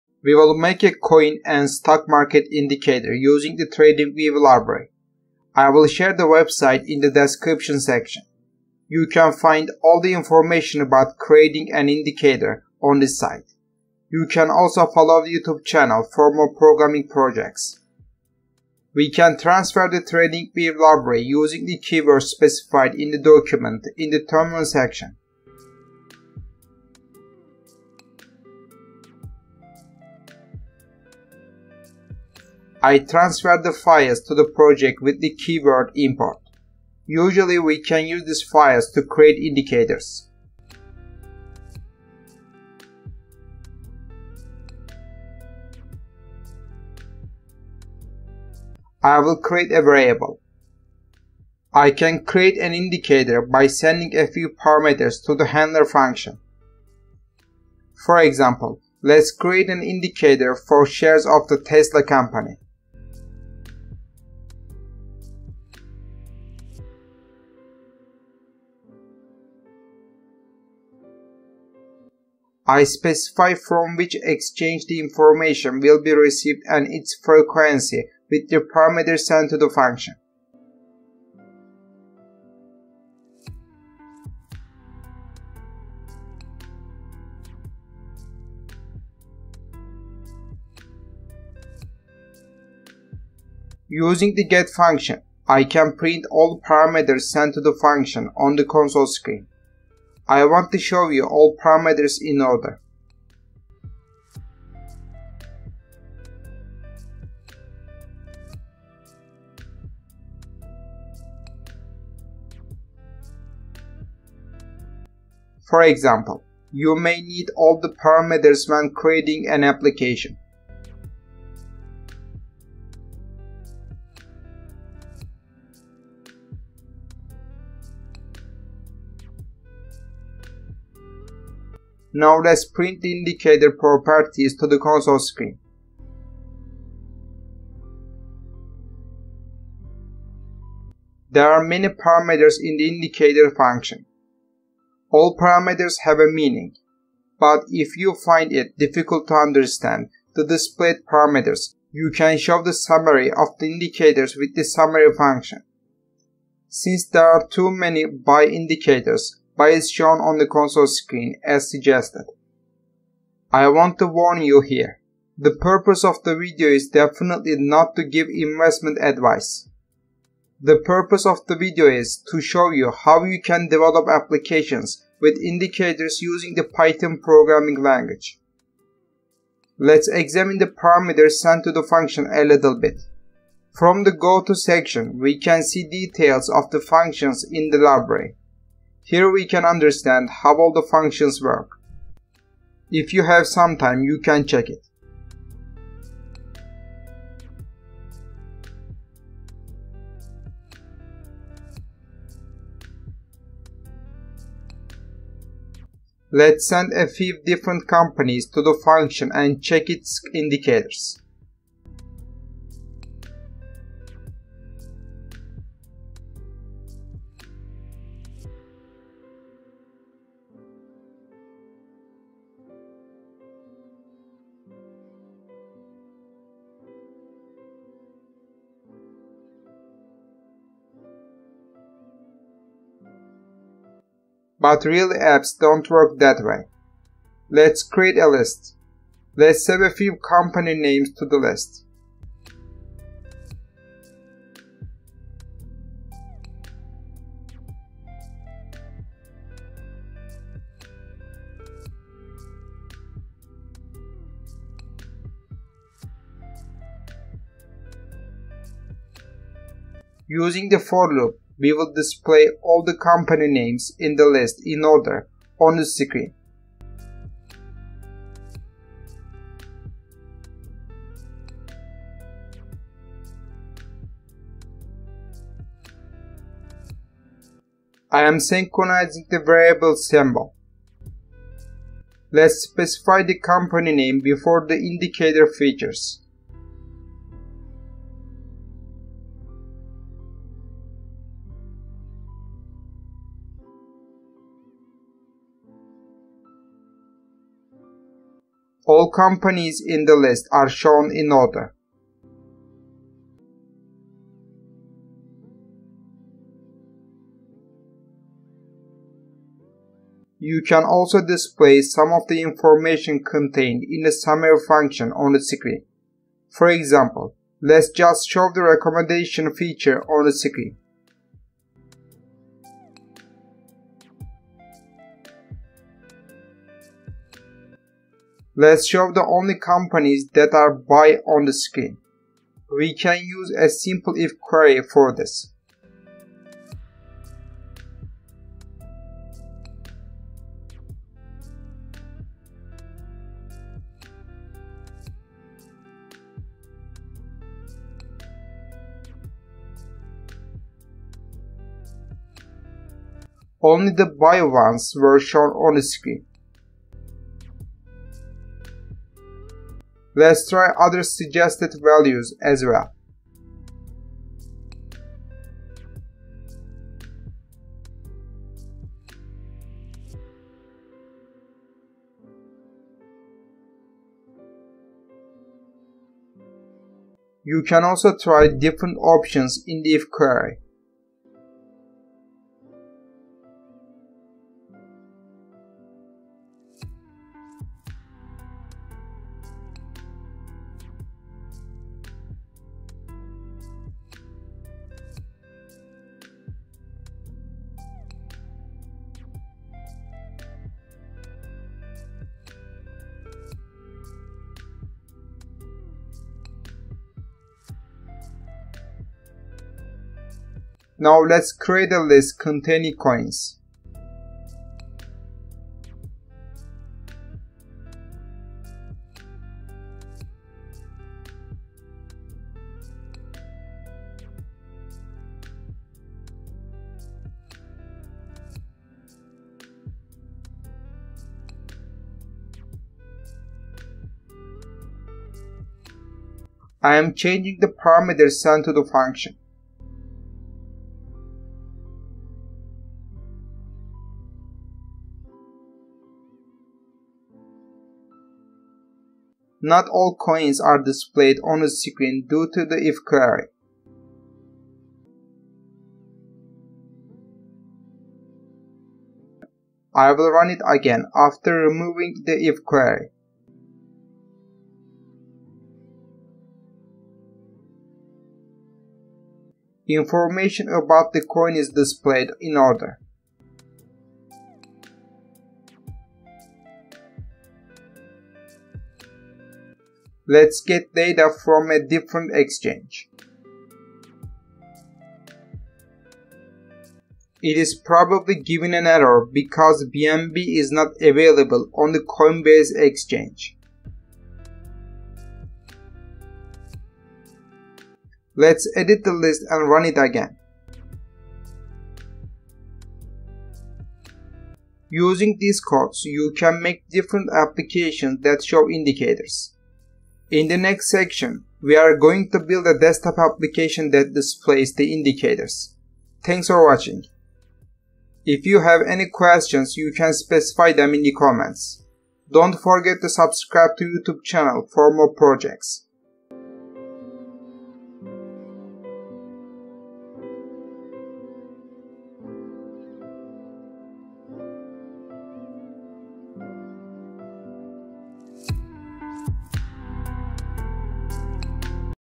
Tradingview Technical Indicator Using Python - PART 1
Let's Create Tradingview Technical Indicator Applications Using Python Programming Language.

Did You Know You Can Create Tradingview Technical Indicator Applications With Python? You Can Create Your Own Indicator Applications By Watching The Video To The End. We Will Create An Indicator Desktop Application In The Next Video.